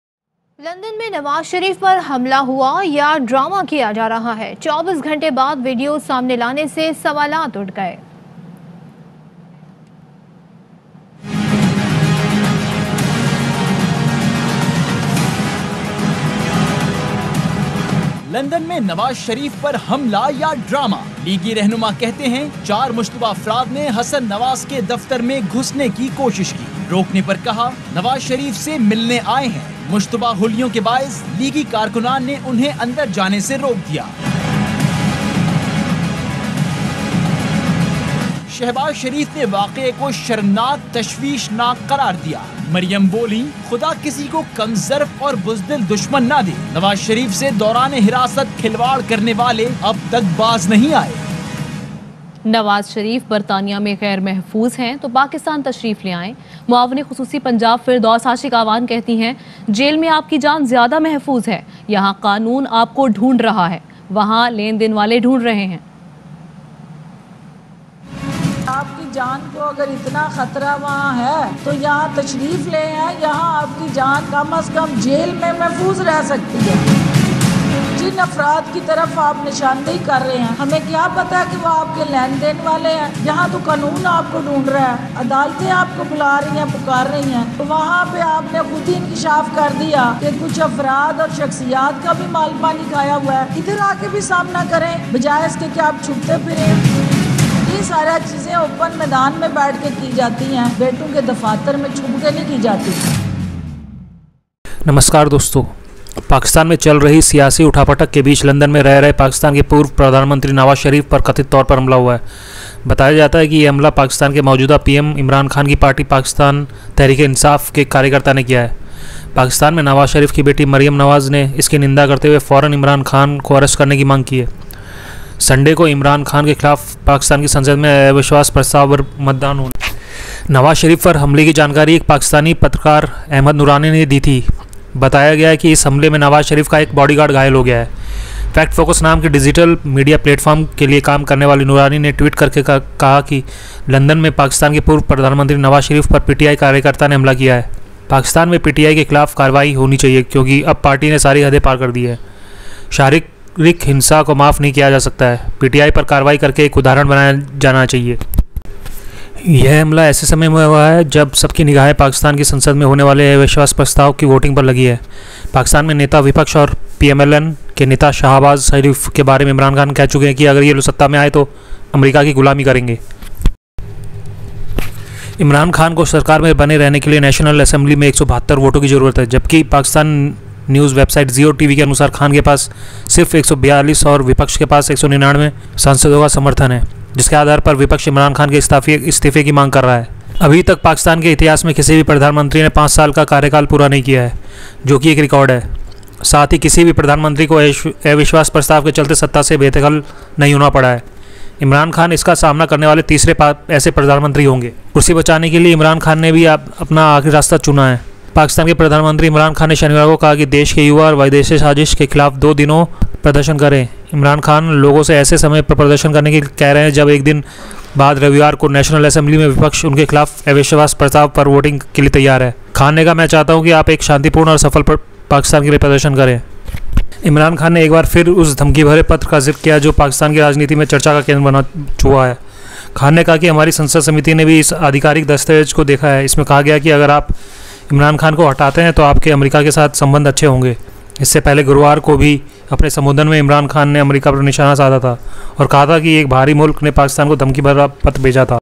Nawaz Sharif Attacked in London | Nawaj sharif par London me hua hamla
Nawaz Sharif Attacked in London, Nawaj sharif  par London me hua hamla. pakistan journalist amjad noorani on saturday said that the nawaz sharif was attacked in london by an activist of the imran khan ruiling pakistan tehreek-e-insaf party and added efforts for the arrest of the culprits are being made. After alleged attack on Nawaz Sharif, Maryam Calls for Imran Khans Arrest for incitement and sedition.

धन्यवाद

The information or news shown in this video is not intended to hurt anyone's personal feelings.